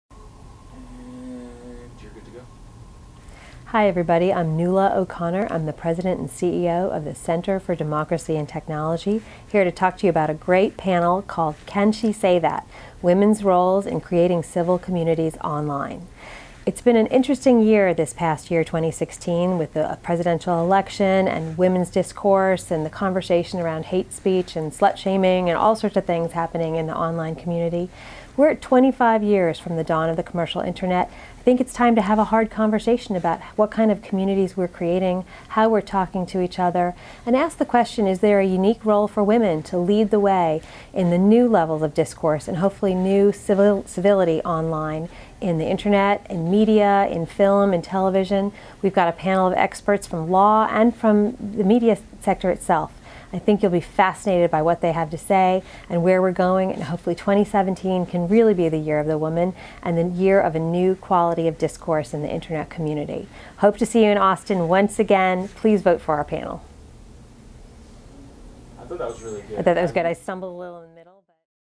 Nuala O'Connor, Women & Civility Online – SXSW 2017, CDT PanelPicker Submission
As more attention is brought to the harassment of women & minorities on online platforms, this all-star, all-female panel will explore and answer: What can be done to do better?

This and more is what Nuala and others are hoping to address in Austin at the upcoming SXSW 2017.

–​ Is there a unique role women can play in creating community, building on historic & cultural norms in community building offline?
– What are some of the platforms doing to promote the best speech and bring out the best in their community members?
– What role does the media play in reinforcing norms and behaviors, particularly towards women?​